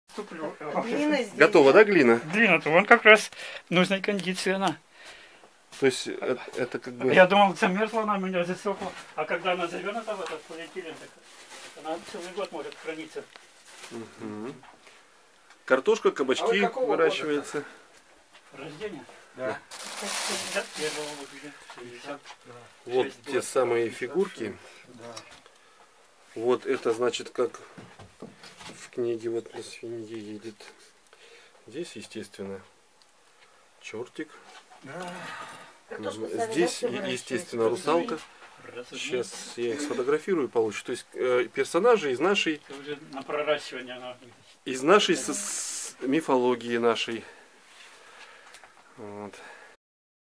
Костромской поэт - отшельник Сергей Потехин. Глина готова
В гостях у Сергея Потехина